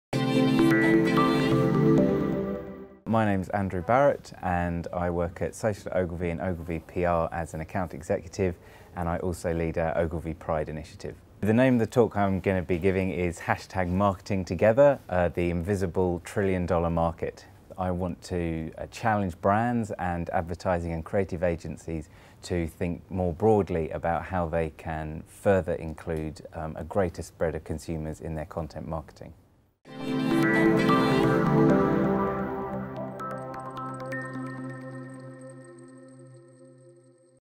Andrew Barratt
Andrew Barratt, Founder and Head of Ogilvy Pride, introduces his talk at Digital Shoreditch: MarketingTogether - the invisible trillion dollar market, exploring why the creative industries needs to embrace diversity and take a more proactive approach to LGBT representation.